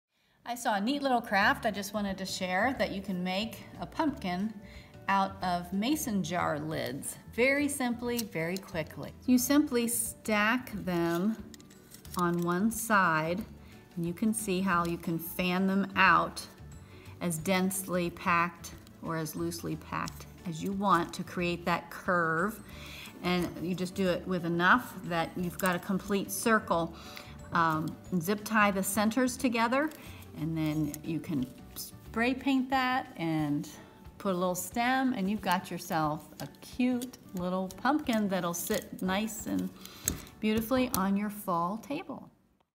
MAKING a “pumpkin” from mason jar lids
Here’s a fun little craft, Making a pumpkin from Mason jar lids. Fun